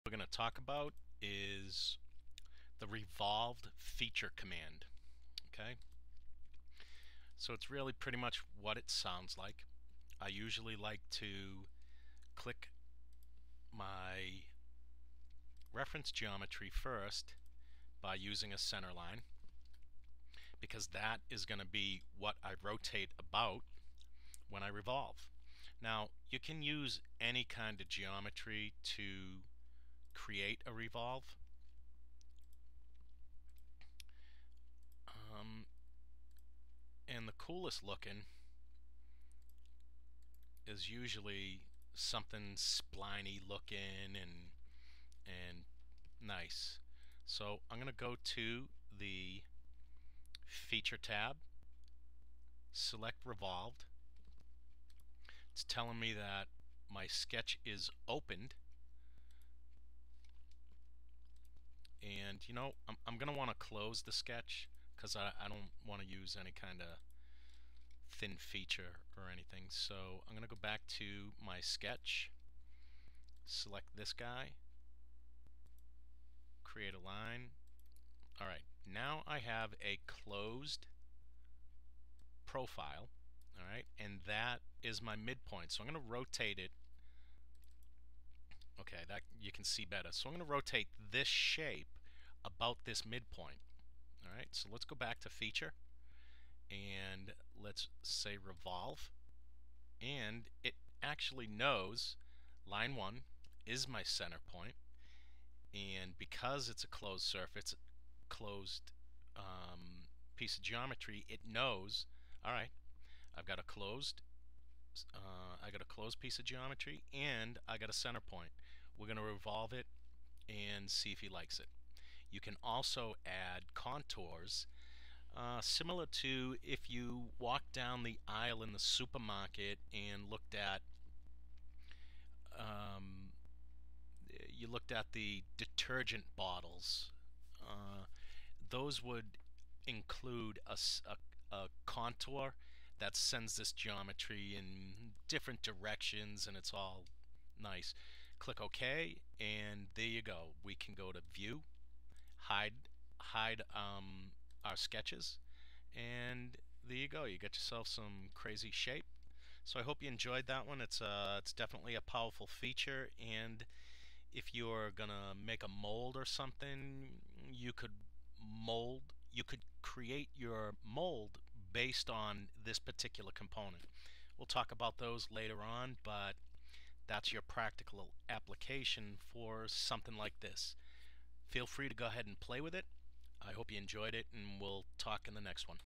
Solidworks Tutorial, Features Revolve command
for the latest Solidworks Video Tutorials Free!
Solidworks Tutorial demonstrating the use of revolve Feature.